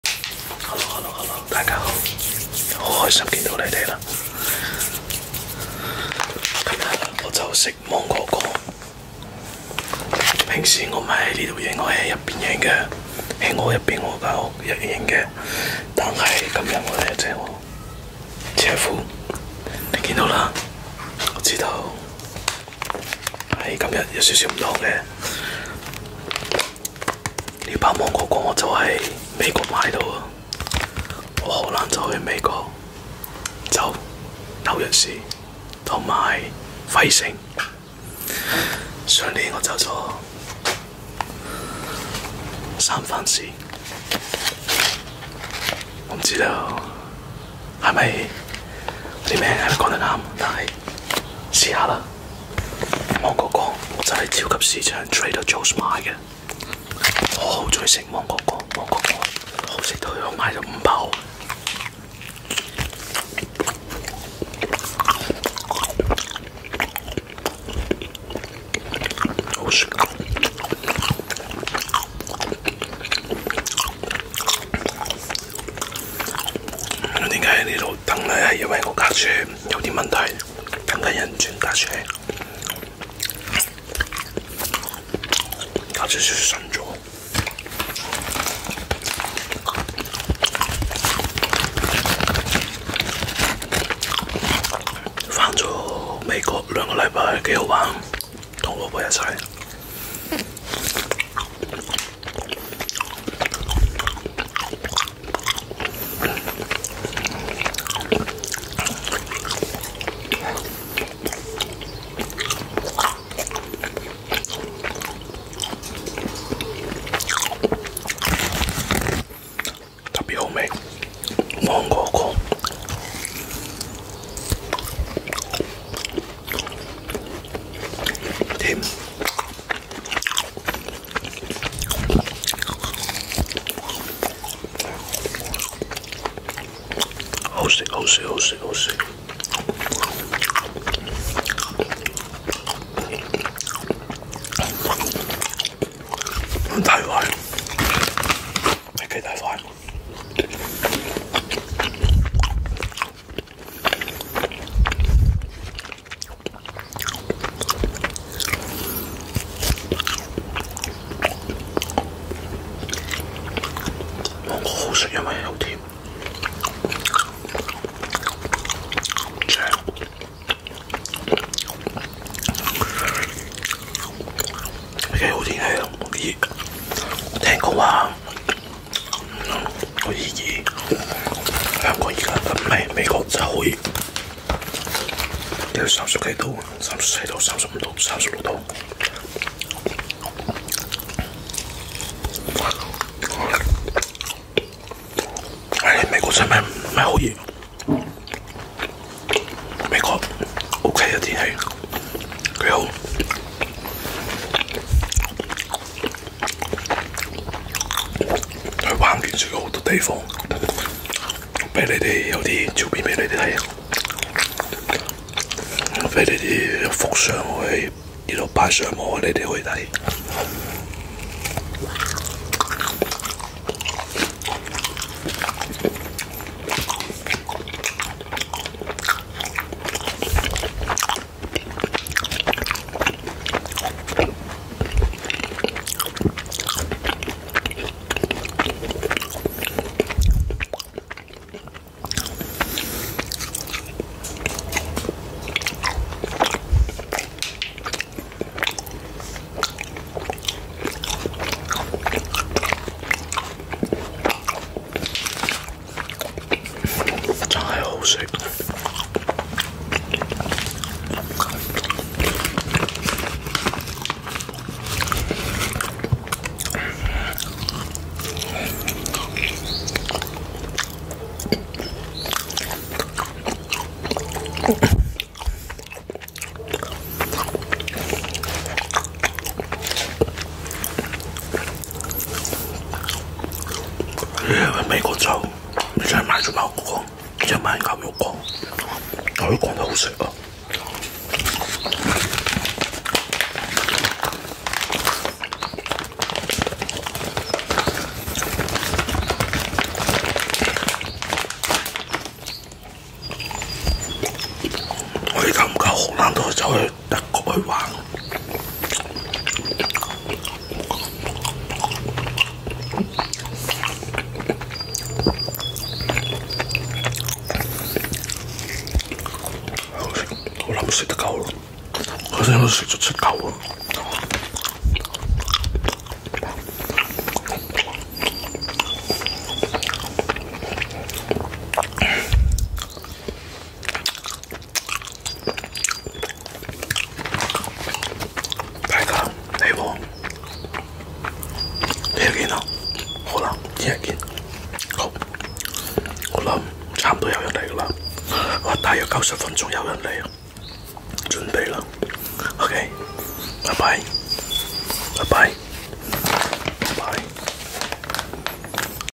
CHINESE ASMR - Dried Mango Eating Sounds 🥭 [ENG Subtitles]
In this Chinese ASMR video I'm eating dried mango to produce eating sounds.